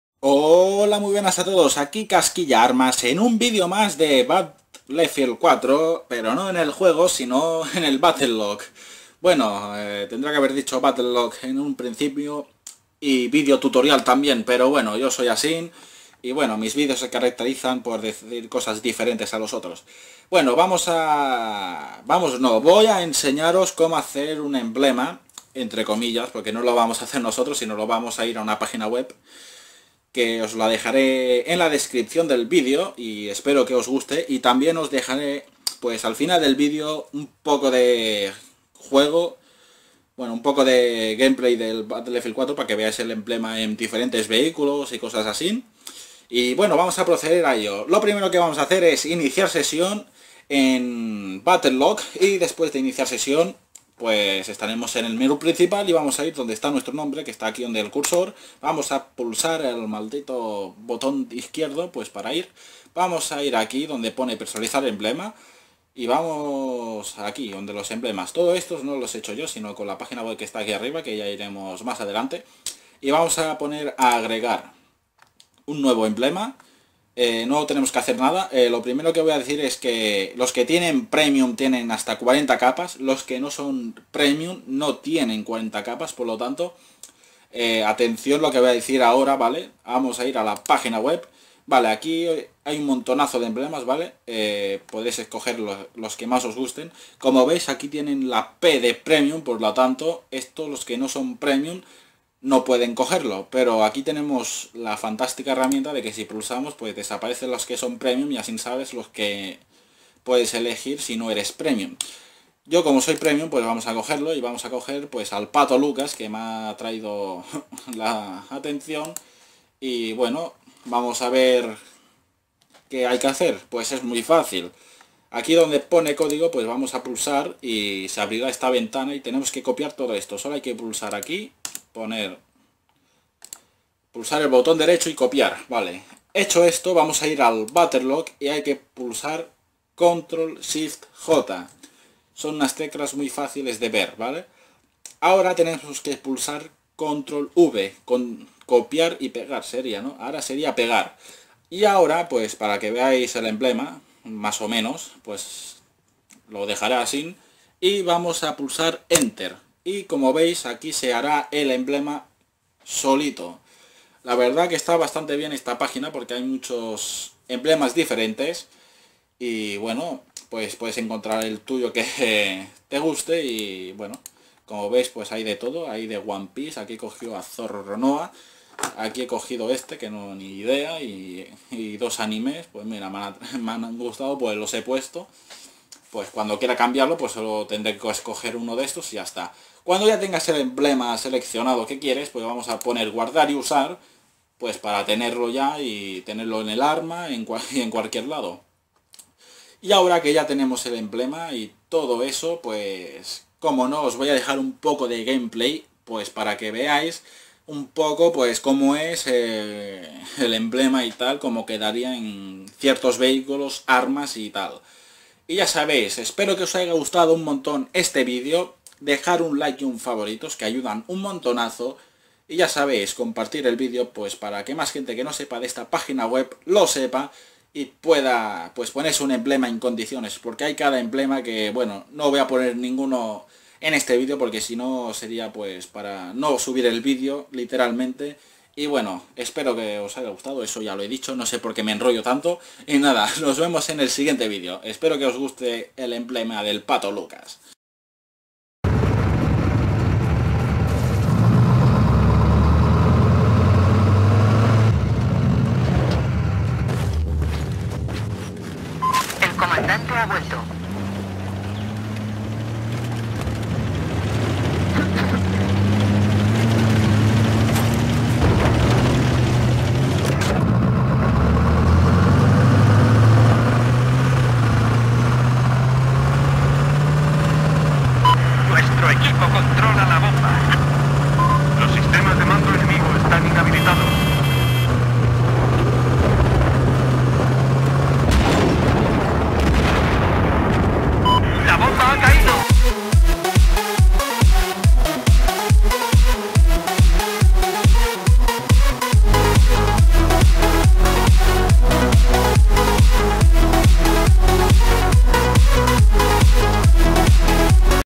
||TUTORIAL DE COMO PONER UN EMBLEMA PERSONALIZADO EN BATTLEFIELD||
Hola a todos seguro que muchos de vosotros estáis hartos de tener un emblema de battlefield 4 que parece que lo fuera echo un niño de 3 años, pues no te preocupes por que aquí te traigo un vídeo tutorial en que podrás buscar y poner un emblema digamos potente, bonito y  precioso, era para decir algo :P. Aquí os dejo el link donde podréis buscar vuestro emblema que mas os guste :D.